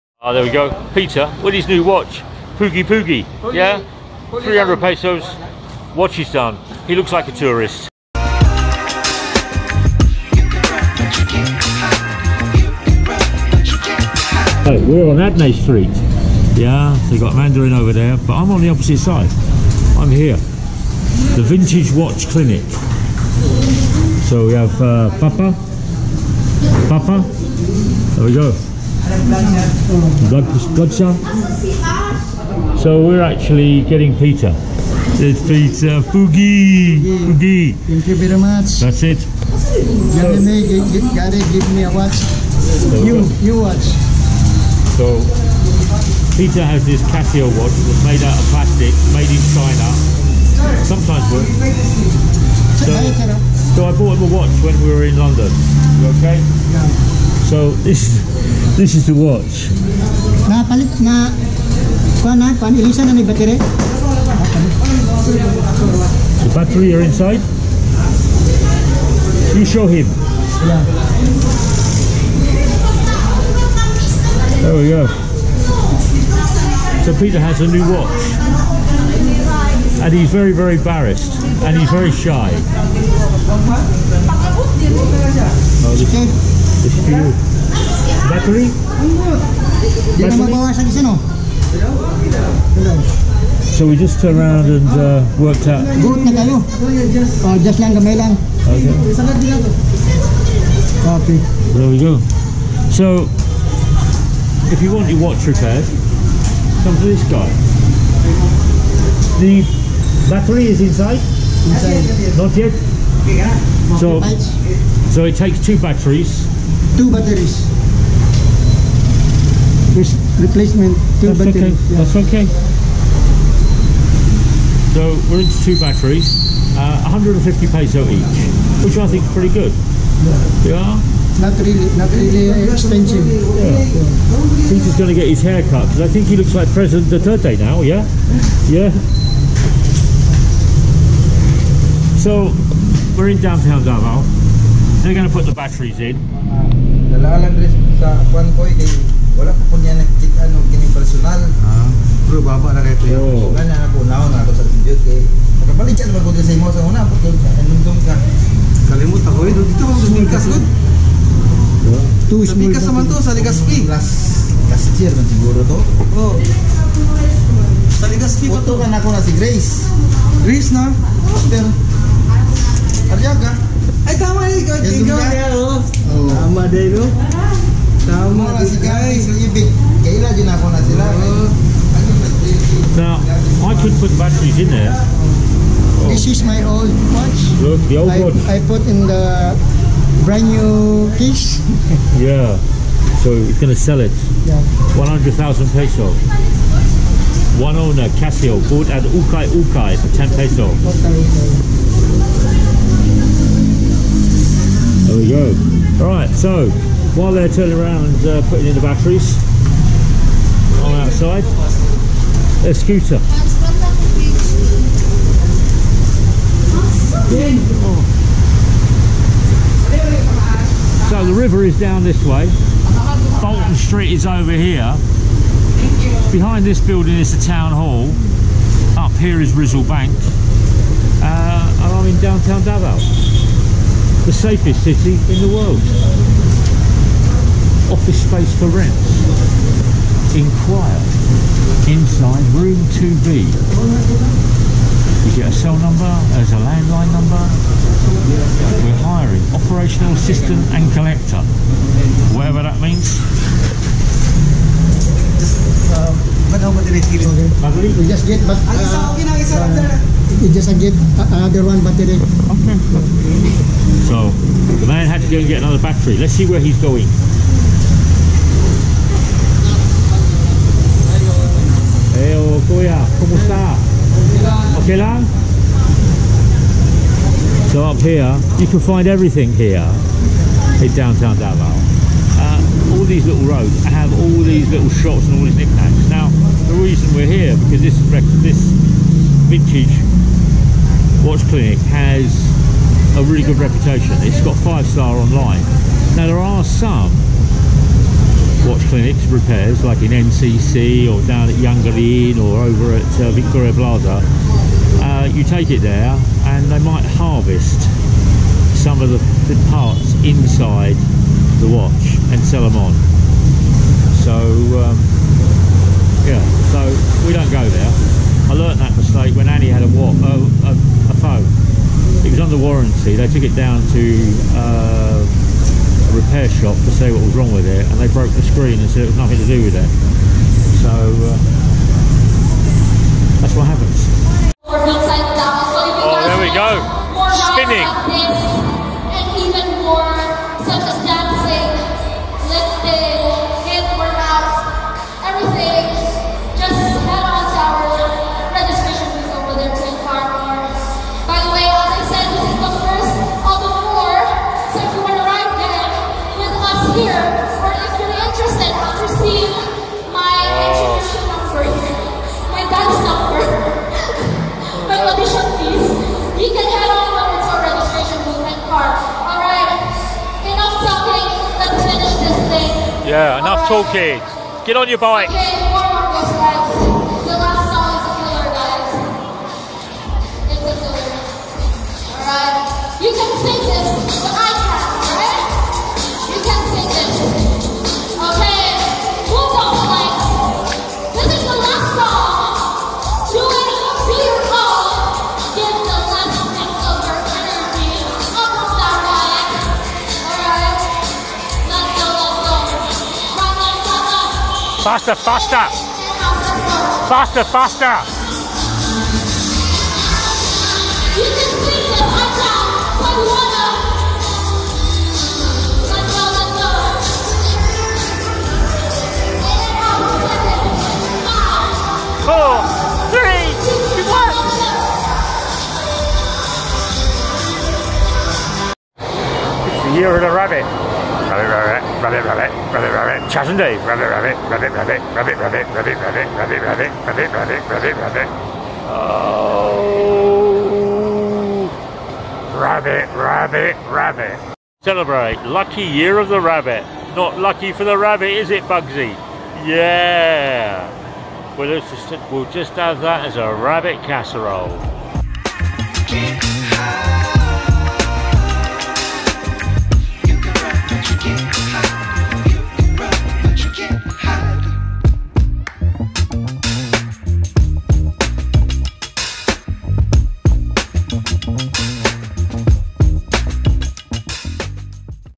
Life Vlog 55 // WHERE TO GET YOUR WATCH REPAIRED DAVAO CITY ph
gifts needs batteries where I go is here..great service can see what they do good knowledge..then off to spinning classes..sweat up at Maoo philippknes *Davao city sholljng lifd is here